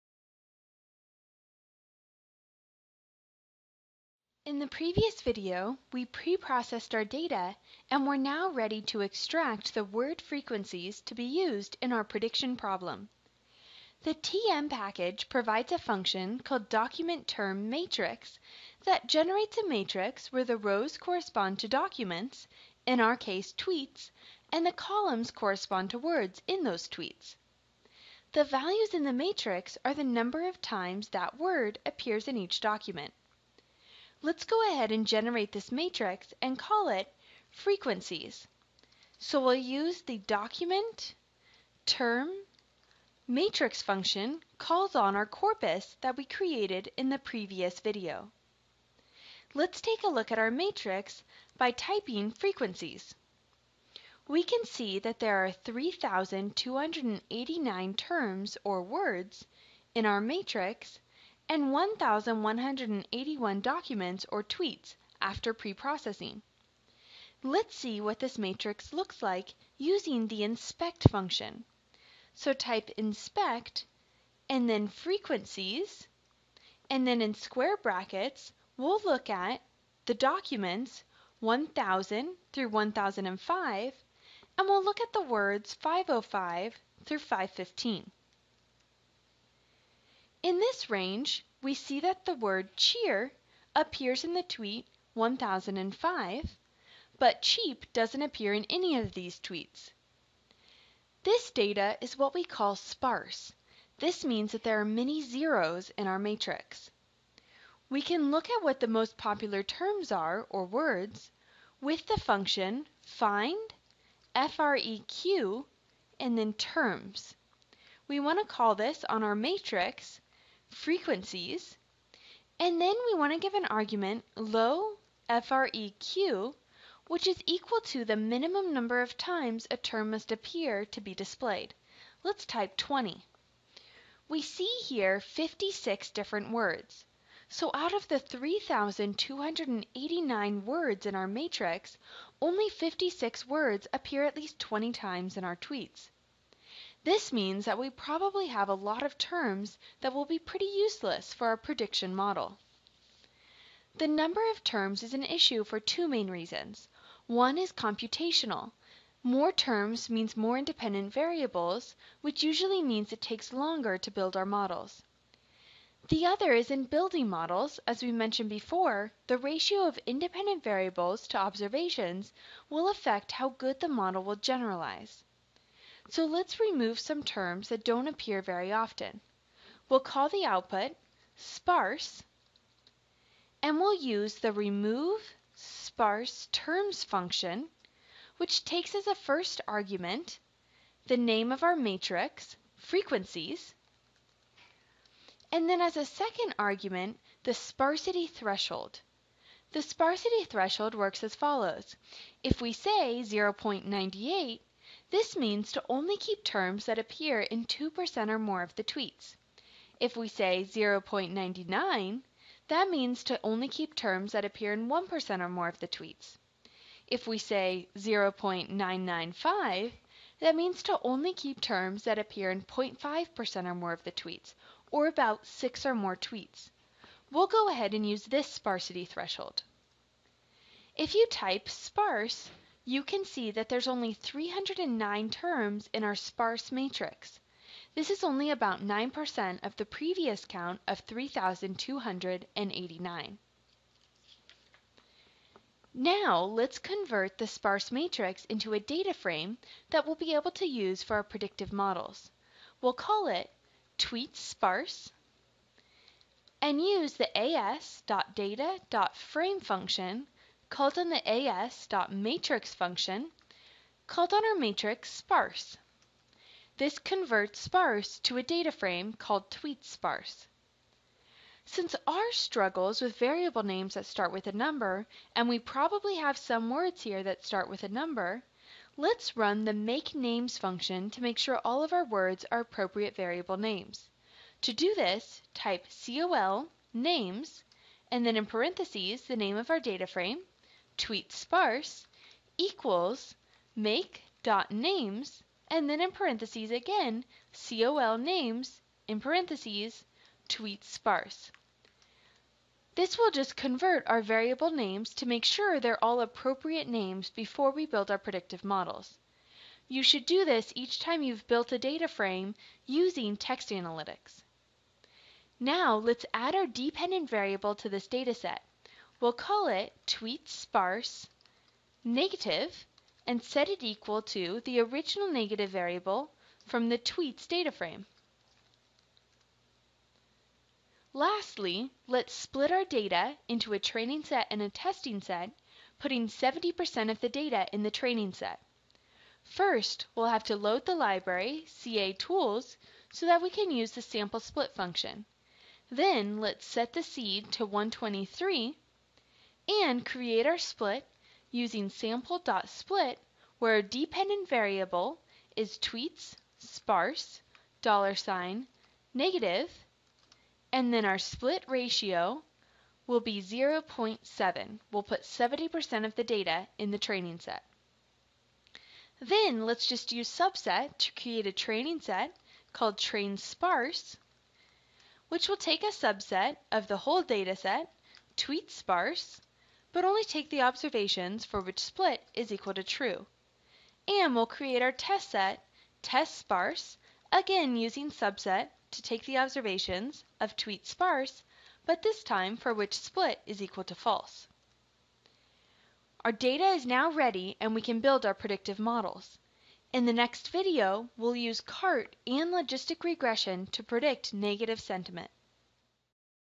5.2.10 An Introduction to Text Analytics - Video 6: Bag of Words in R
Extracting the word frequencies to be used for the prediction problem.